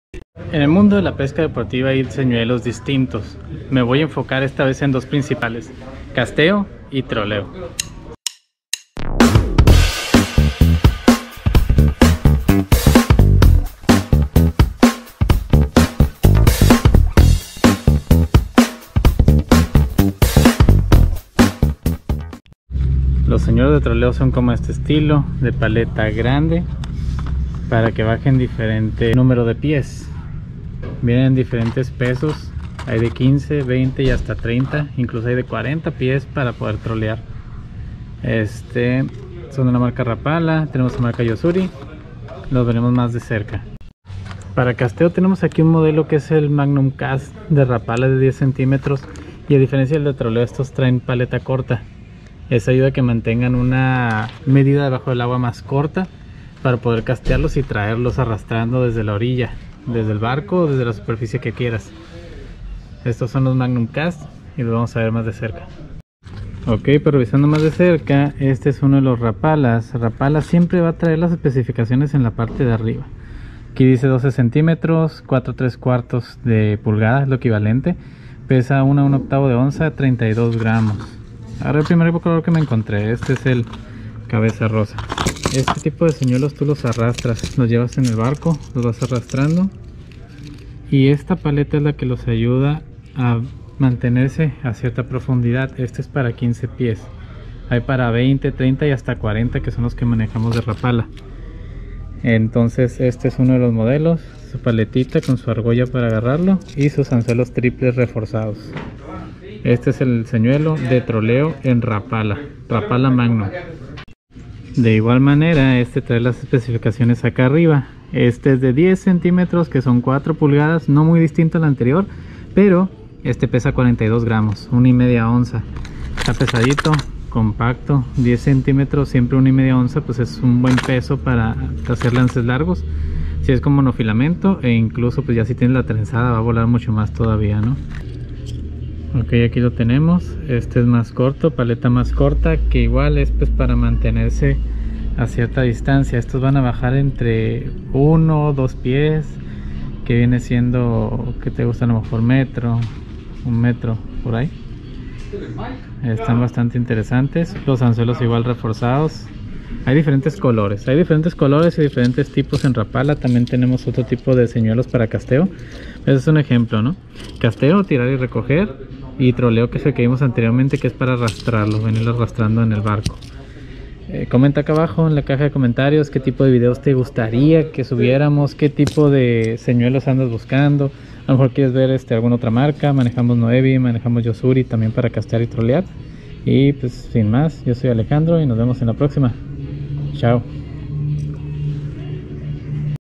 SEÑUELOS DE PESCA RAPALA, TROLEO Y CASTEO ¿CUAL ES CUAL?
Señuelos de Pesca Rapala, te presentamos dos de sus modelos, uno para troleo y otro para castreo!!!